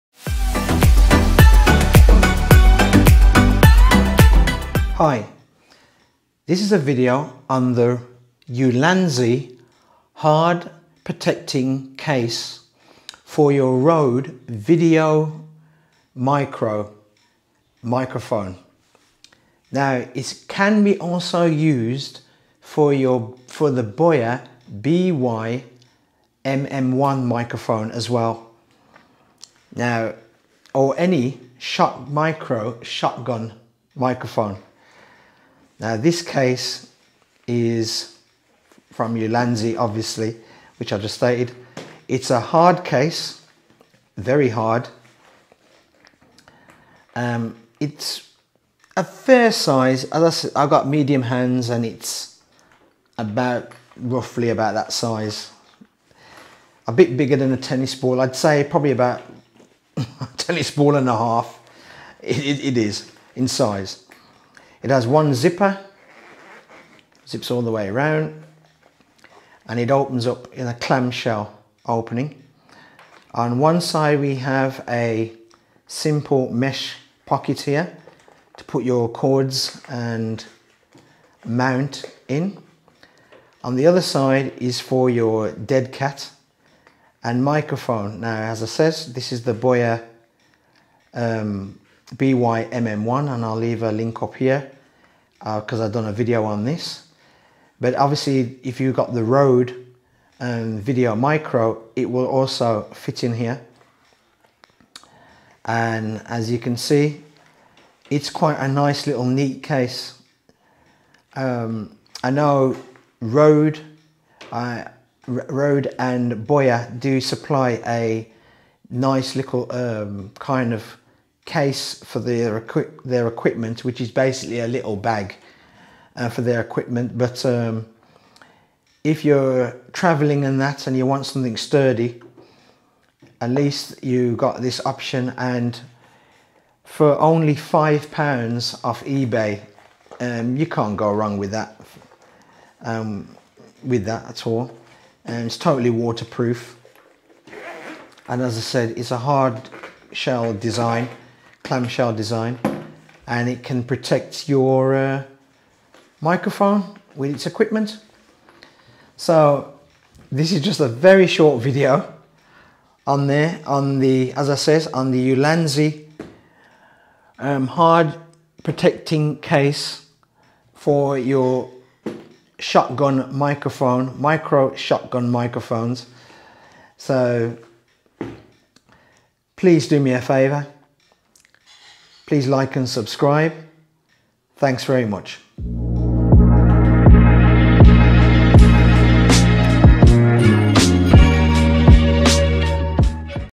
Ulanzi AriMic Protective Case QUICK REVIEW
Ulanzi AriMic Protective Case Portable Box Hard Travel Carrying Cover Box for RODE Video RODE VideoMic Me microphone

Special Designed for RODE VideoMicro & VideoMic Me Microphones,BOYA-MM1
BEST Case to Protect your Microphone
Also fit for Sony ECMXYST1M Stereo Microphone
Size: 14*9*4.5cm